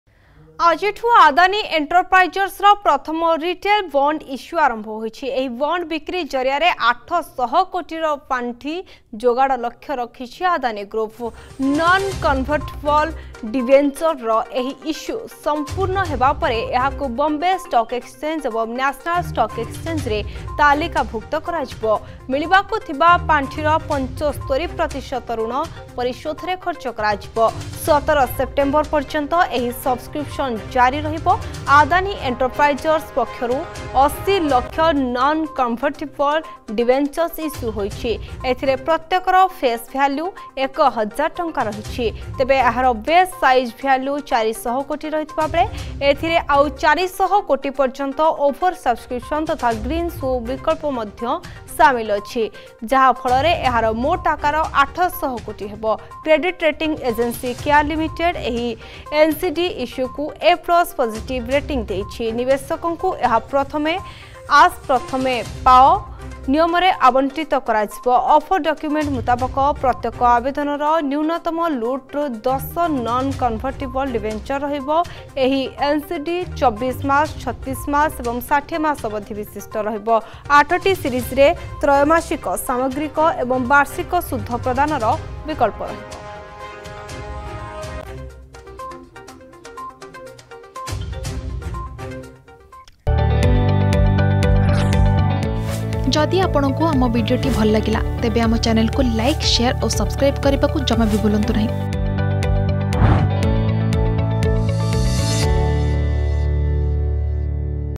ଆଜିଠୁ ଆଦାନୀ ଏଣ୍ଟରପ୍ରାଇଜର୍ସର ପ୍ରଥମ ରିଟେଲ୍ ବଣ୍ଡ ଇସ୍ୟୁ ଆରମ୍ଭ ହୋଇଛି || Adani Group || Bond
ଆଜିଠୁ ଆଦାନୀ ଏଣ୍ଟରପ୍ରାଇଜର୍ସର ପ୍ରଥମ ରିଟେଲ୍ ବଣ୍ଡ ଇସ୍ୟୁ ଆରମ୍ଭ ହୋଇଛି । ଏହି ବଣ୍ଡ ବିକ୍ରି ଜରିଆରେ ୮୦୦ କୋଟିର ପାଣ୍ଠି ଯୋଗାଡ଼ ଲକ୍ଷ୍ଯ ରଖିଛି ଆଦାନୀ ଗ୍ରୁପ । ନନ-କନଭର୍ଟିବଲ୍ ଡିବେଞ୍ଚରର ଏହି ଇସ୍ୟୁ ସଂପୂର୍ଣ୍ଣ ହେବା ପରେ ଏହାକୁ ବମ୍ବେ ଷ୍ଟକ୍ ଏକ୍ସଚେଞ୍ଚ ଏବଂ ନ୍ୟାସନାଲ୍ ଷ୍ଟକ୍ ଏକ୍ସଚେଞ୍ଜରେ ତାଲିକାଭୁକ୍ତ କରାଯିବ ।